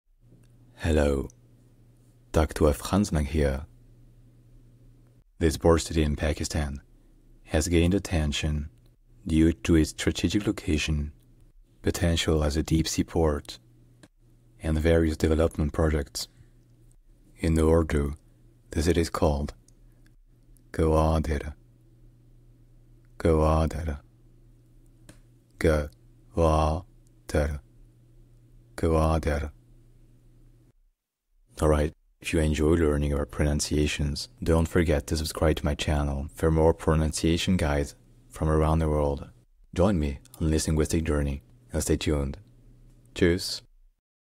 🇵🇰 How to pronounce Gwadar
How do you say 'Gwadar'?

Gwadar, a port city in Pakistan, has gained attention due to its strategic location, potential as a deep-sea port, and various development projects connected to the China-Pakistan Economic Corridor (CPEC).